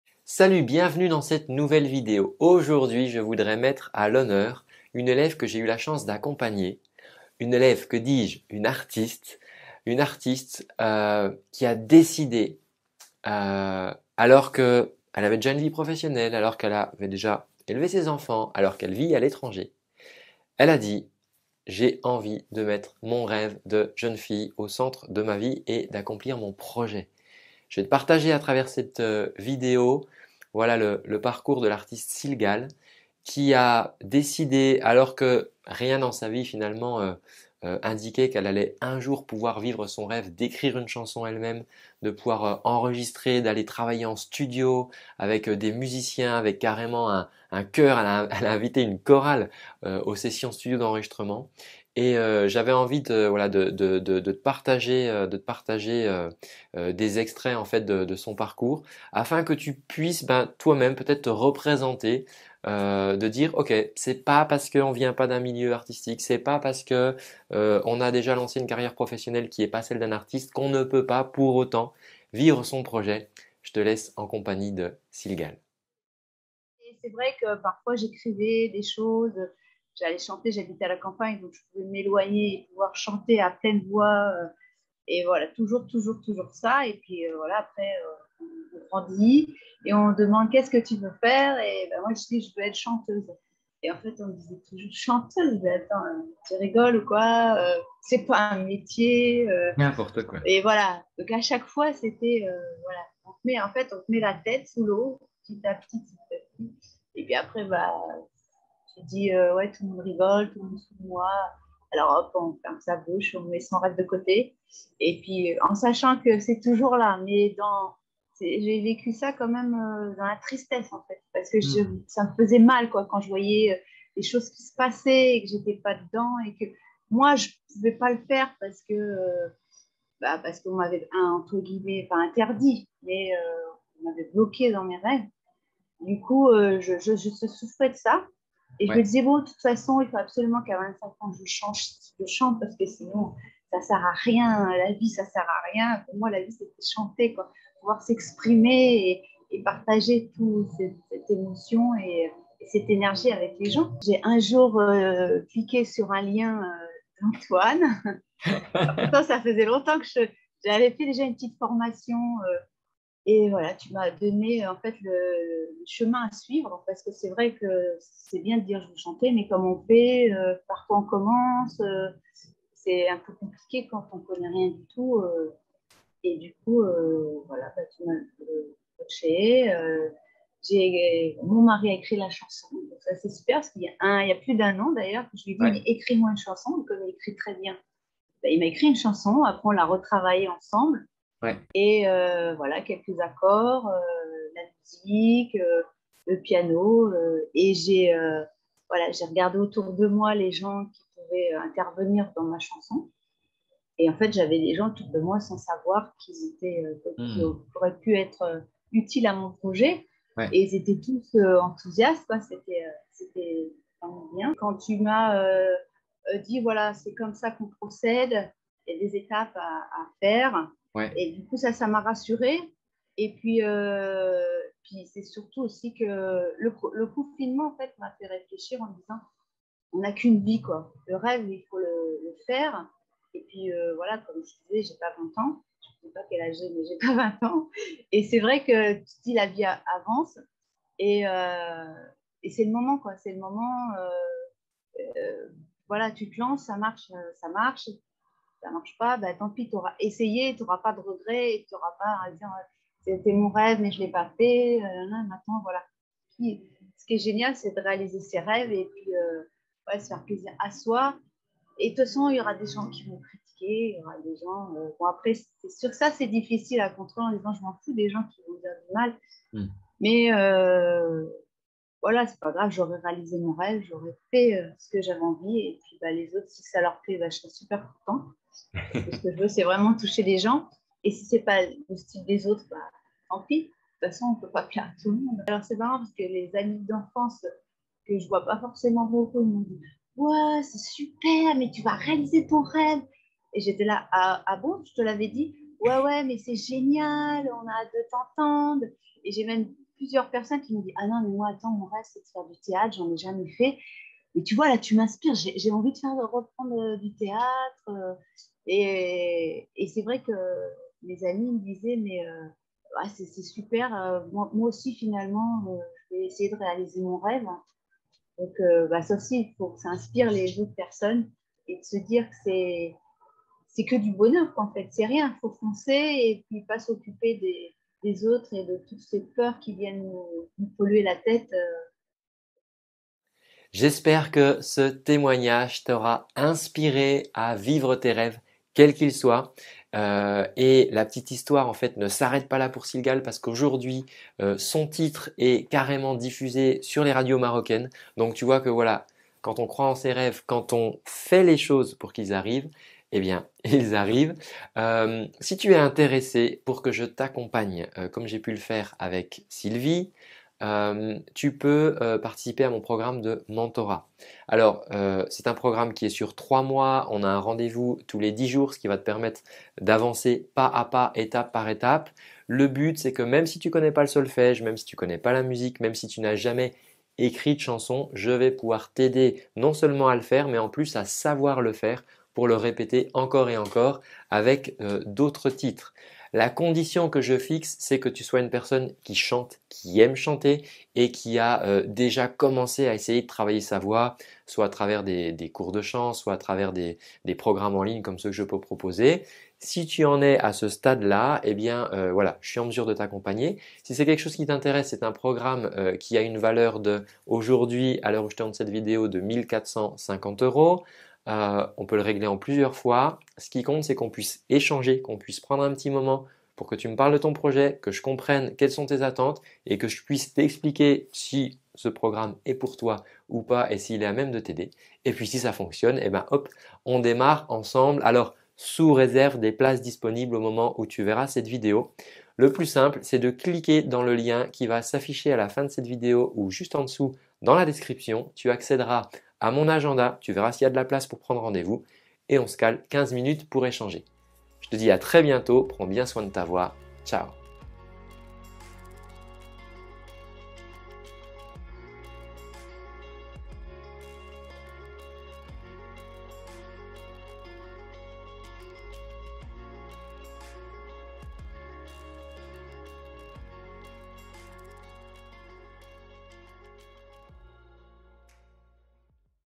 Témoignage : Elle réalise son rêve d'adolescente
👉🏻 Si tu veux que je t'accompagne comme j'ai pu le faire avec Sylgal

Aujourd'hui je voulais laisser la parole à une autre chanteuse avec qui j'ai eu un immense plaisir de travailler.
Il y a quelques mois encore, elle pensait qu'avec sa carrière professionnelle, sa vie de maman, elle devait renoncer à ses rêves. Et pourtant, alors que depuis toute jeune fille elle désespérait que cela puisse arriver un jour, aujourd'hui son premier titre tourne sur les Ondes Marocaines.

Récemment Sylvie m'a appelé pour me dire qu'elle rentrait en studio pour son 2ème titre !

J'espère que ces nouvelles te font entrevoir qu'il toujours possible de vivre son rêve si on y croit encore et qu'on s'en donne les moyens.

Le résultat ?